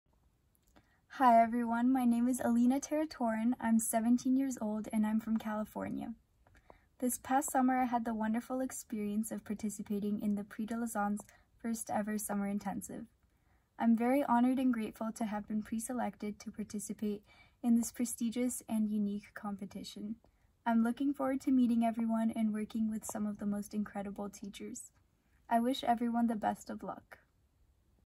Alina TARATORIN - 17 years old - United States
🎬 Video selfies of the Prix de Lausanne 2020 candidates ⭐